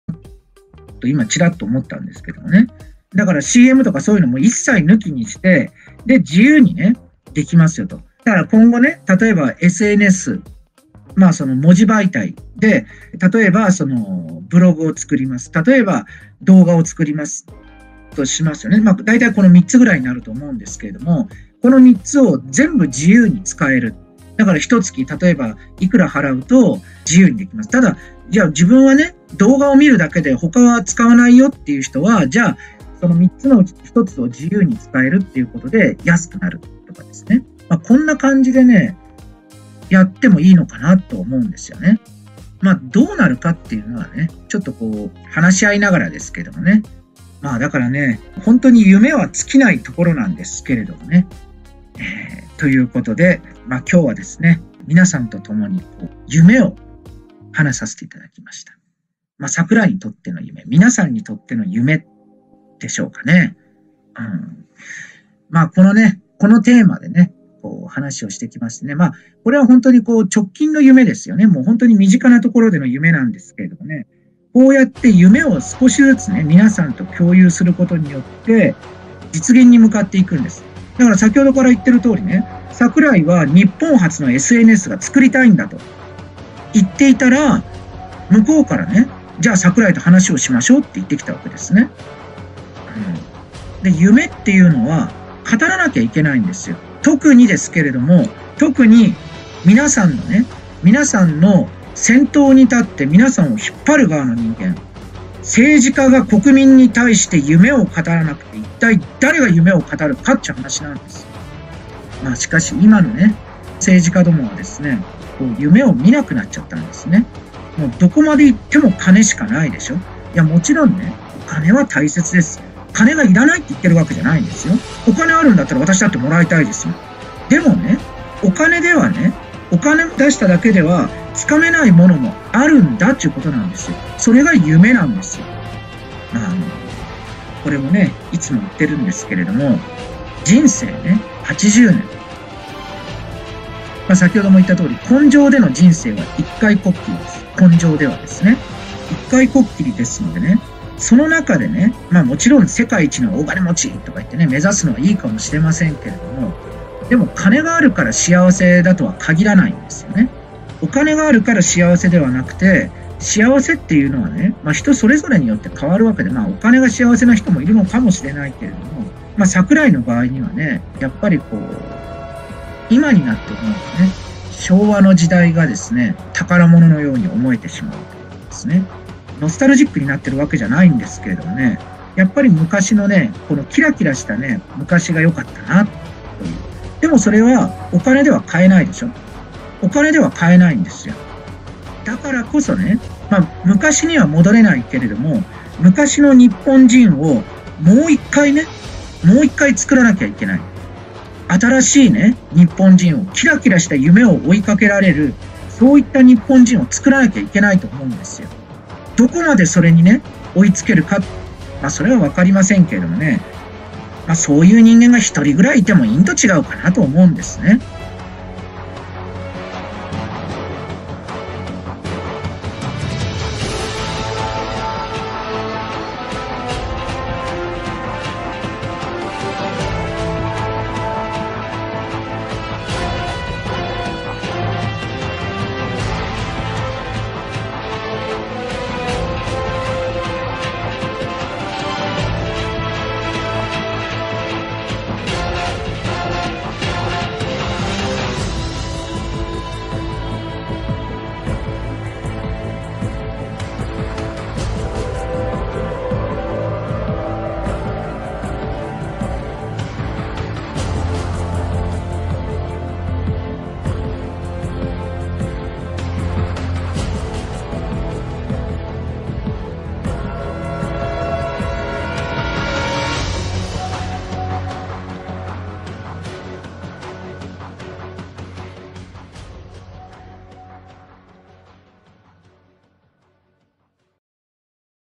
桜井誠先生が夢の挑戦？ 日本人だけの為のＳＡＳ。 ツイッター・フェイスブックの終焉。 『続き編』
桜井誠先生が夢の挑戦？　日本人だけの為のＳＡＳ。

しかし二つのＳＡＳは完全に中韓型となり特に日米保守団体はごとごとく凍結された。
そして最近はフェイスブックではあかんと削除システムを発動させこれもまたに渡米保守同時の削除大多数が起きた。
日本人だけの為のＳＡＳは存在せず日本人としては不便な思いをして来た中で果たしてどんなＳＡＳが誕生するのか？
これからの夢を持つ日本の未来の為に。

お隣の国は消して味方だと思うな。

前の動画が最後のシーンが途切れたので続き編として追加いたしました。